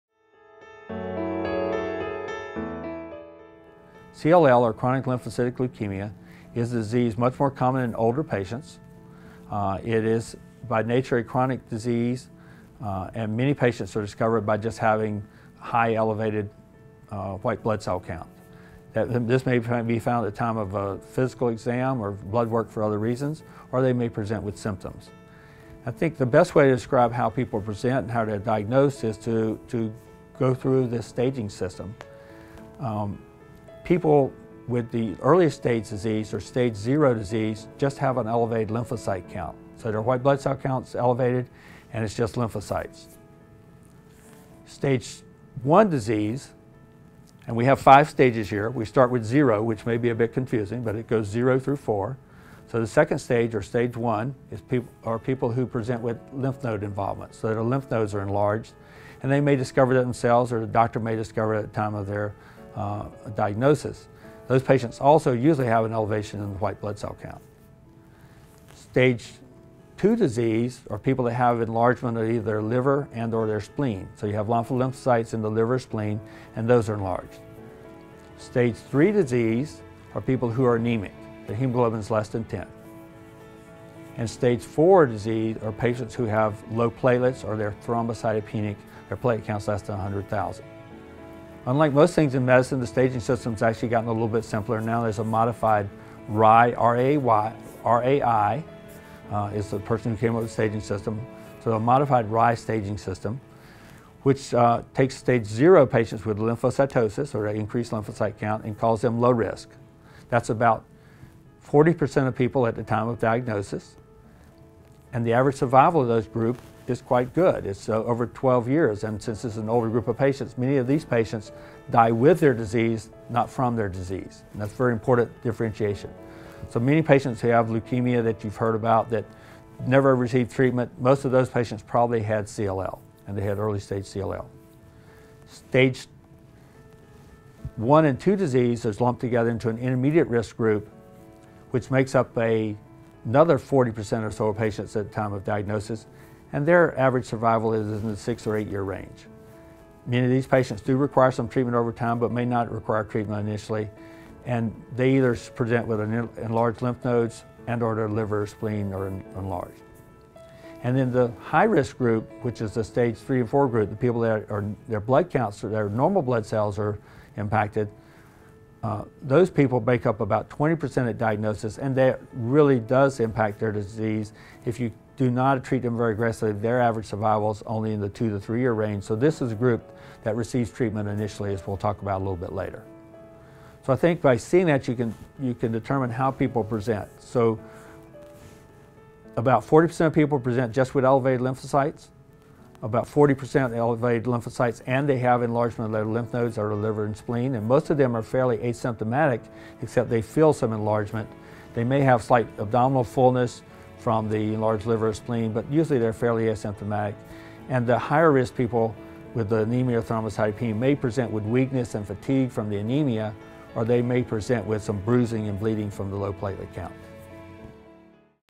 Staging of Chronic Lymphocytic Leukemia
A talk about the Staging of Chronic Lymphocytic Leukemia by Dr. Bayard L. Powell, M.D. of Wake Forest Baptist Health for Cancer Guide Posts of Strength.